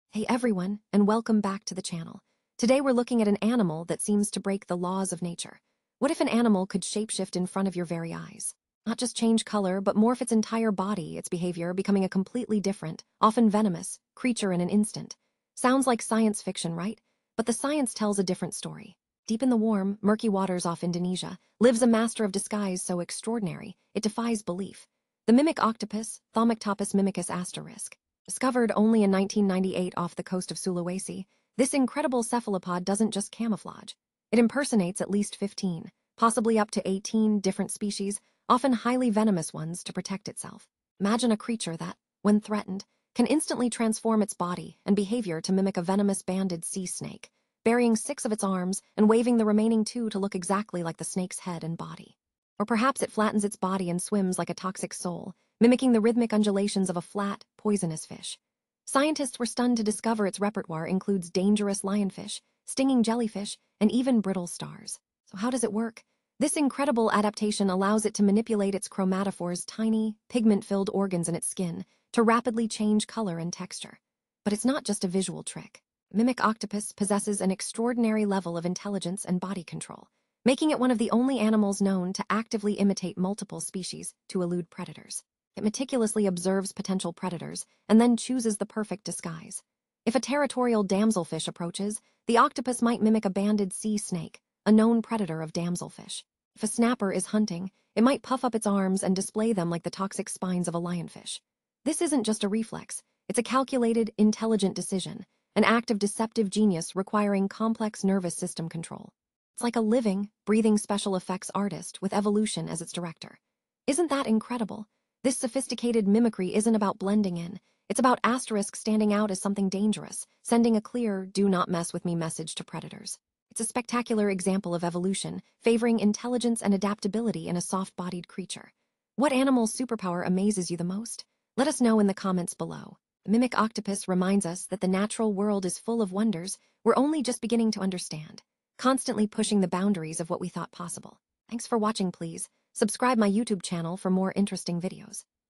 Mimic Octopus: Is This Shapeshifting Legend REAL & ALIVE?
Across forgotten scrolls and whispered tales, humanity has always wondered about creatures that defy belief – shapeshifters capable of becoming anything. But what if one such legendary beast wasn't just a myth? Dive deep with us as we uncover the astonishing truth behind the Mimic Octopus, a creature so extraordinary it blurs the line between mythology and reality. For centuries, ancient legends spoke of beings that could transform at will. Today, cryptozoology might have found its most compelling proof. The Mimic Octopus is not merely camouflage; it's an intelligent impersonator, mimicking venomous sea snakes, lionfish, and even flatfish with terrifying accuracy. Is this legendary creature actually alive and thriving, exhibiting abilities once thought impossible? We explore the evidence, dissect the mystery, and reveal the incredible truth. Could this be the ultimate real shapeshifter, haunting the ocean depths? Join us to witness the astonishing footage and decide for yourself! [0:00] The Shapeshifter Enigma [2:15] Ancient Tales & Modern Sightings [4:30] Unbelievable Mimicry REVEALED [7:00] Is the Mimic Octopus the Ultimate Proof? What do you think - real or myth?